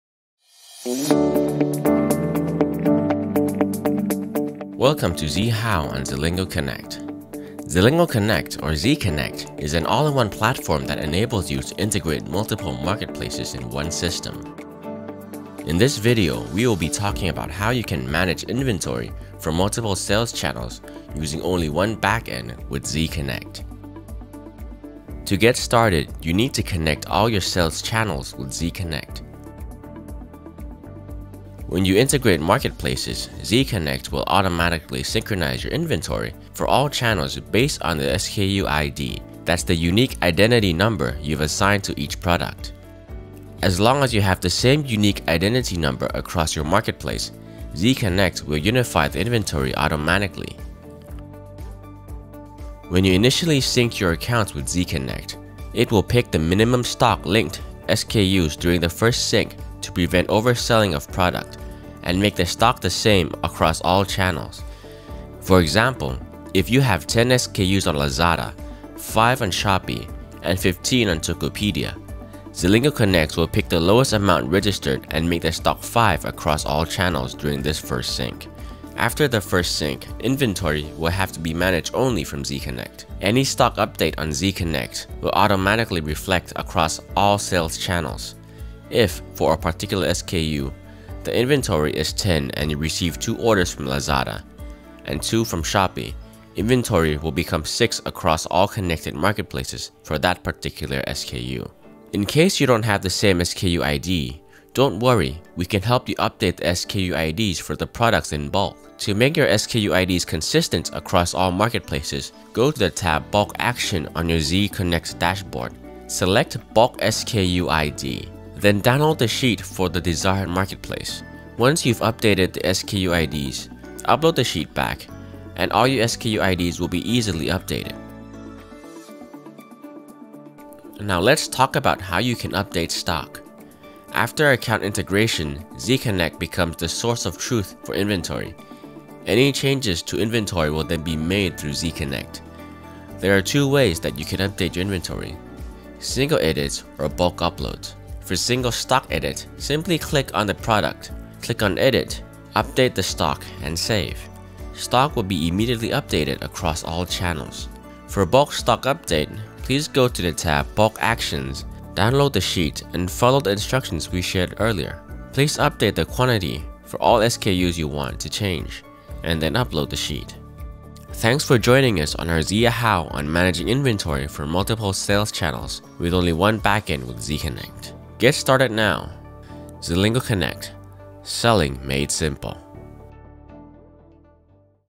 Zilingo Connect - Inventory Management Tutorial | English Version
Introducing Zilingo Connect
Making online sales easy!
Zilingo Connect is an all-in-one platform that enables you to seamlessly manage all your online marketplaces.